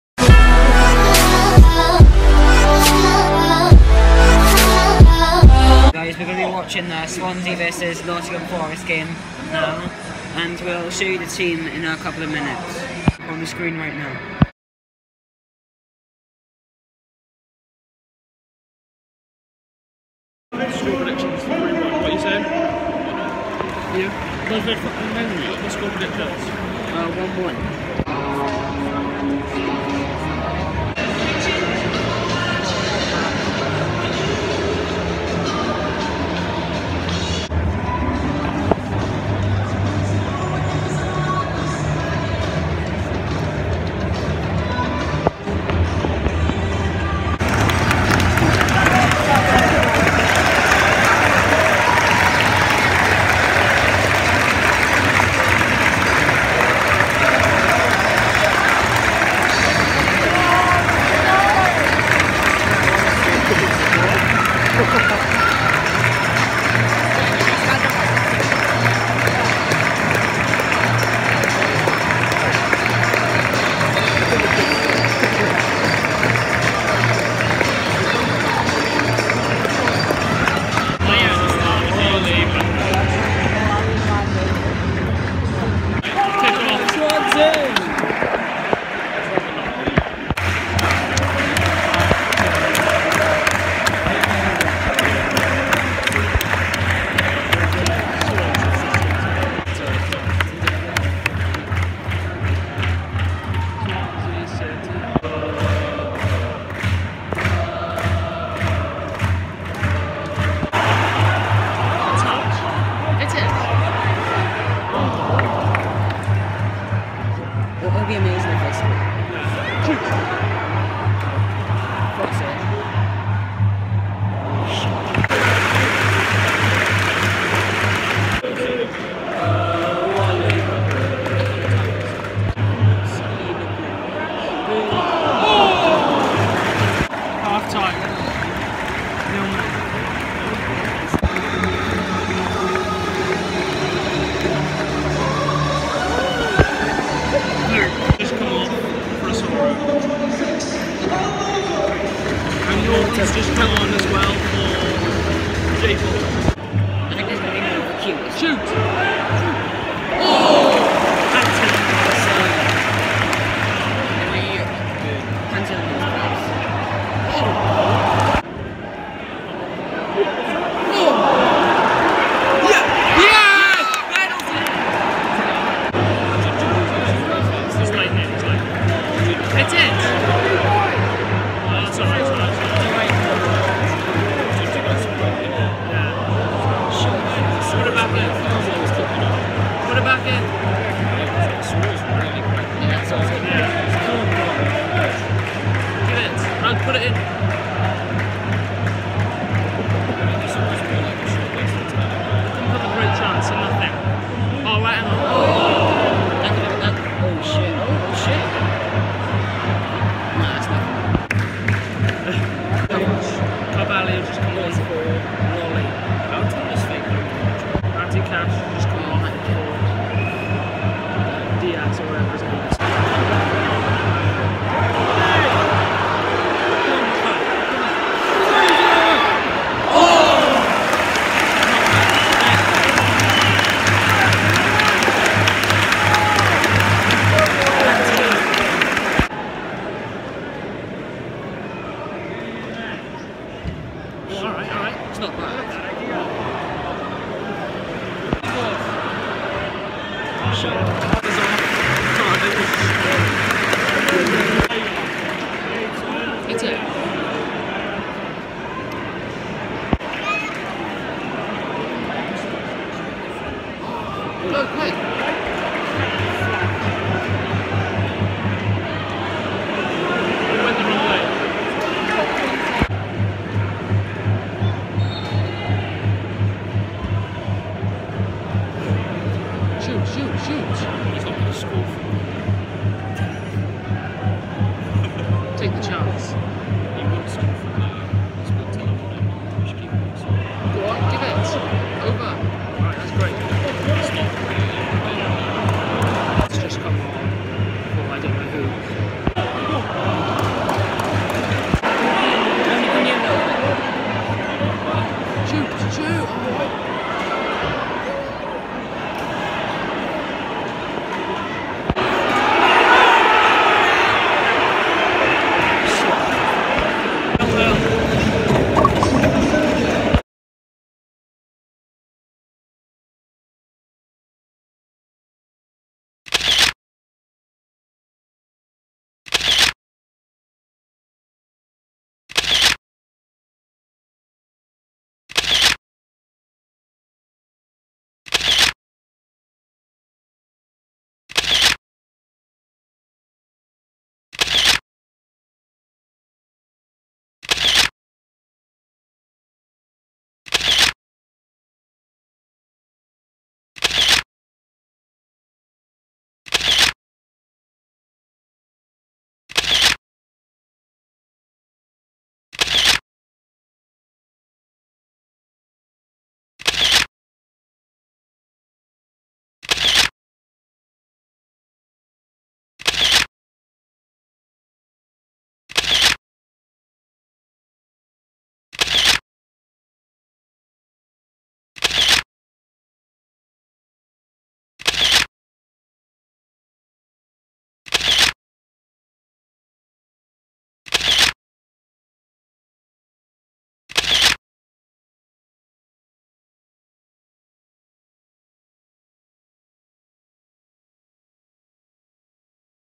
CLASSIC STALEMATE|SWANSEA 0-0 NOTTINGHAM|MATCHDAY VLOG #2
Today Jayke,Luke and Dylan vlog the Swansea City vs Nottingham Forest game in the Championship and we also meet the players.

               ✌️PEACE OUT✌️